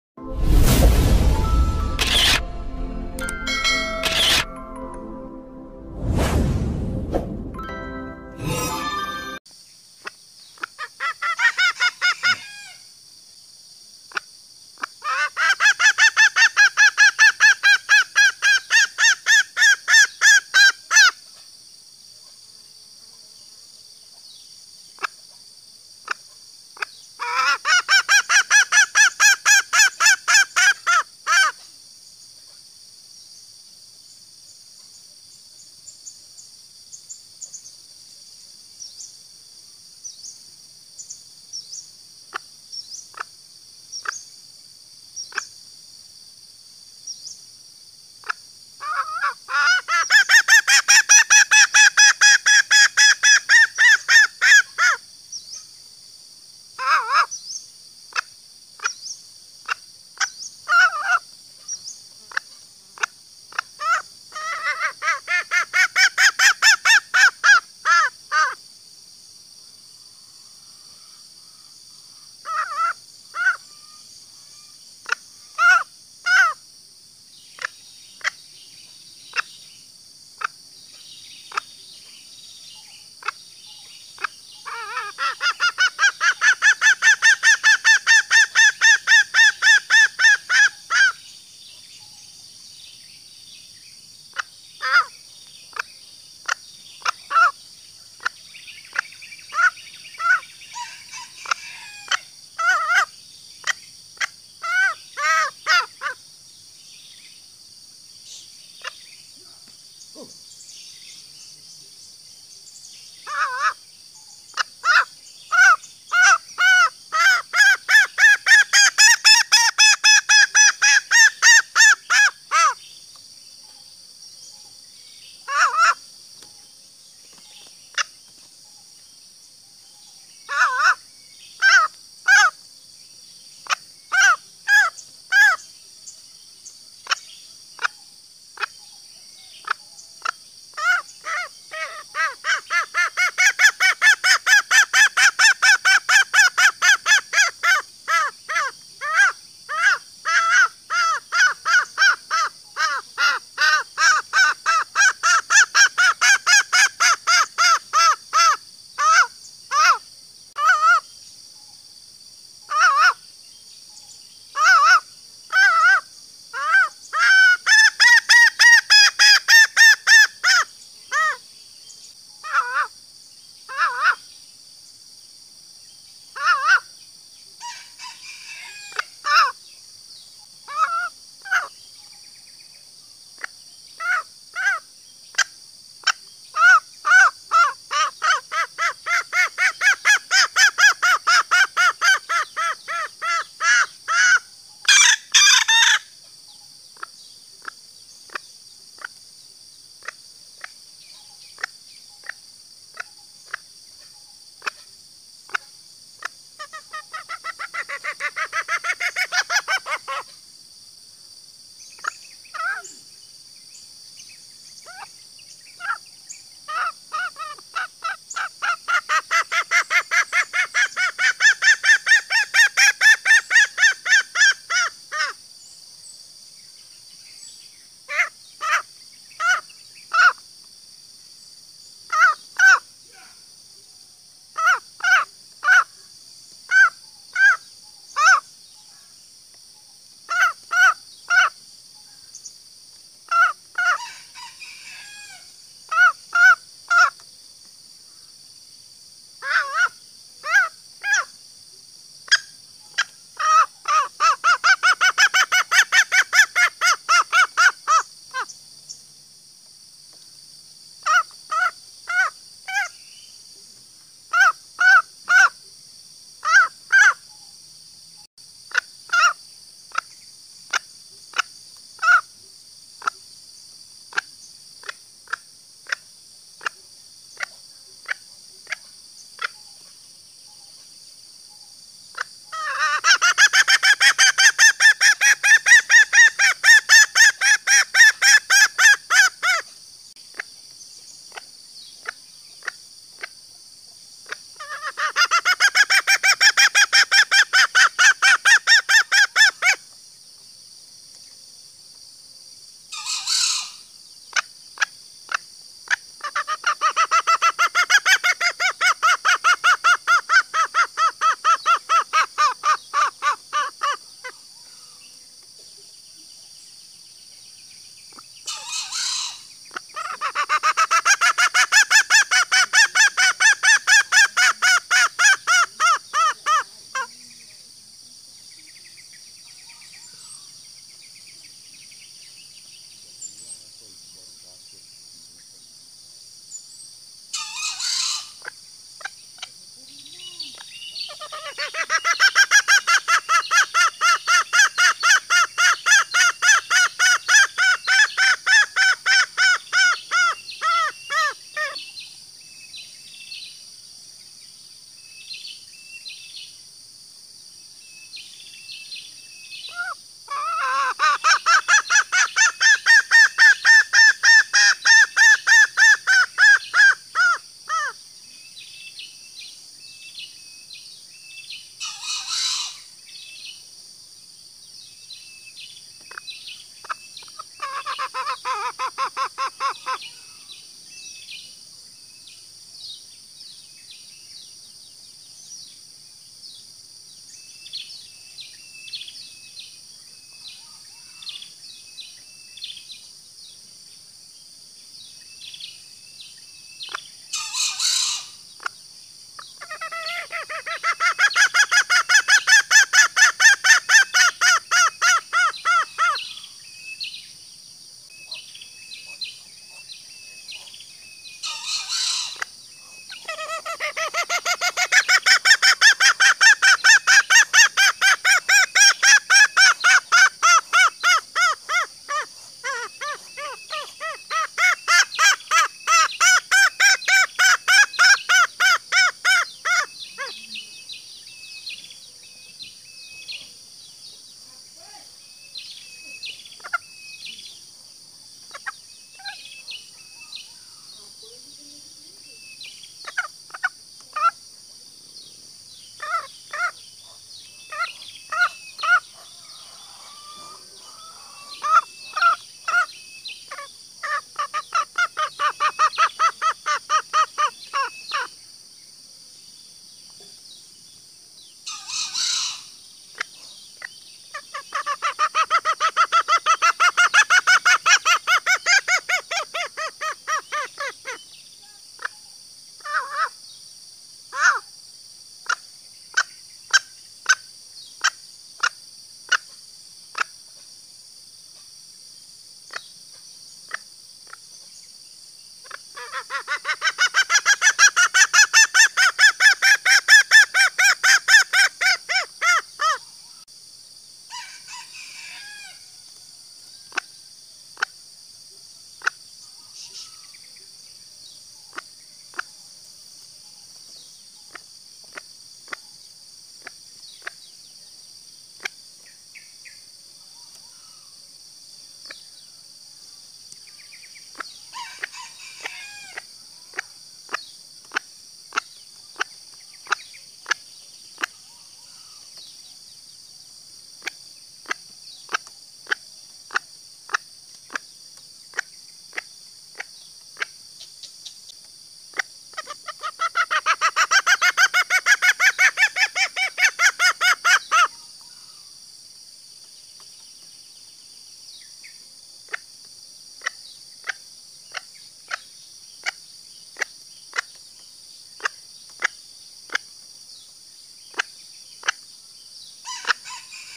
Masteran suara betina ayam hutan hijau super ampuh untuk memancing suara pejantan
Ayam hutan hijau (Latin: Gallus varius) adalah nama sejenis burung yang termasuk kelompok unggas dari suku Phasianidae, yakni keluarga ayam, puyuh, merak, dan sempidan. Ayam hutan diyakini sebagai nenek moyang sebagian ayam peliharaan yang ada di Nusantara. Ayam ini disebut dengan berbagai nama di berbagai tempat, seperti canghegar atau cangehgar, ayam alas ajem allas atau kerate

Memiliki nama ilmiah Gallus varius (Shaw, 1798), ayam ini dalam bahasa Inggris dikenal sebagai Green Junglefowl, Javan Junglefowl, Forktail, atau Green Javanese Junglefowl, merujuk pada warna dan asal tempatnya.
SUMBER: Wikipedia